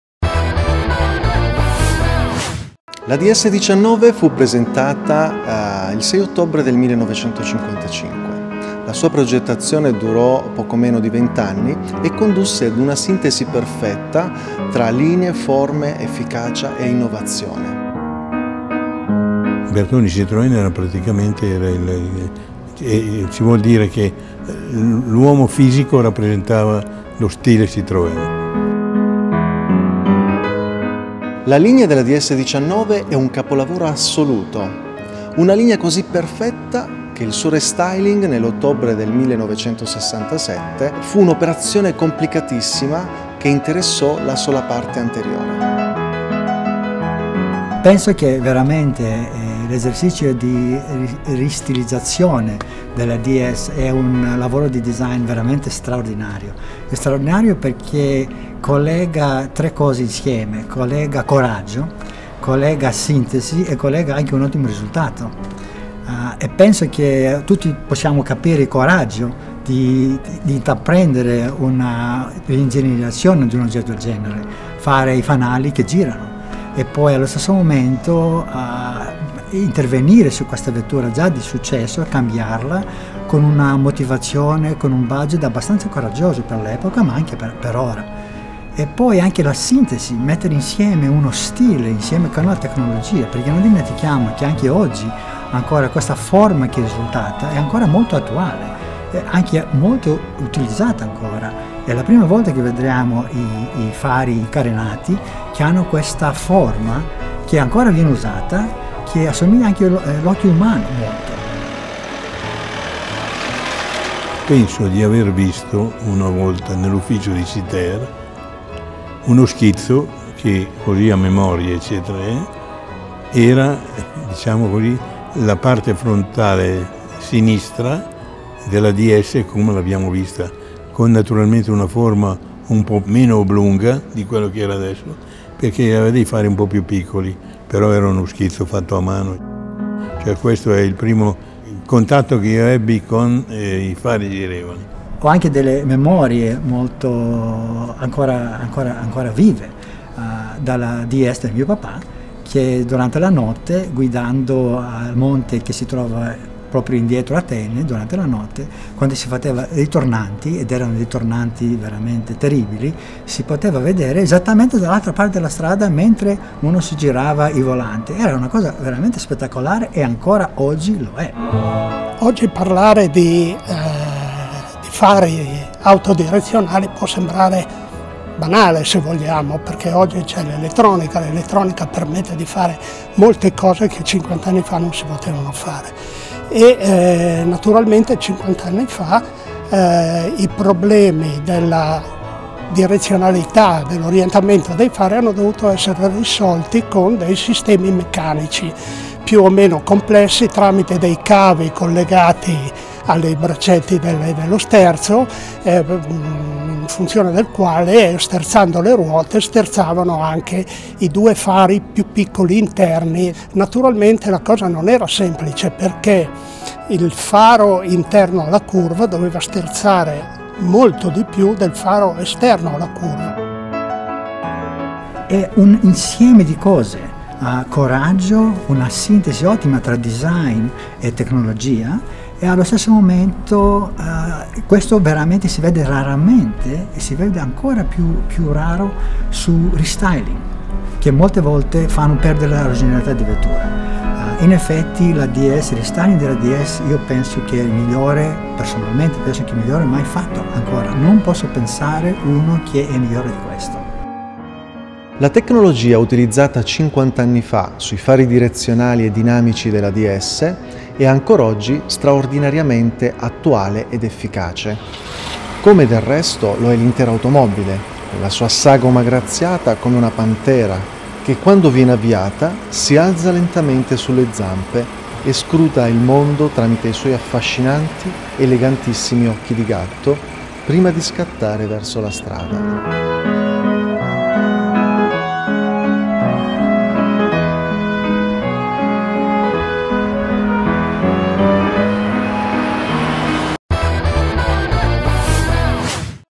NEW TUTORIAL DS: 30 ANNI DI STORIA CON LA DS E I FARI ORIENTABILI
Ismaele Iaconi – Responsabile Comunicazione DS Automobiles – Camillo Saini – Progettista Citroen DS19 – Andreas Zapatinos – Car Designer – e Mario Maiocchi – Centro DocumentazioneStorica Citroen & DS – raccontano e descrivono la DS nella sua originalità nello scorso secolo.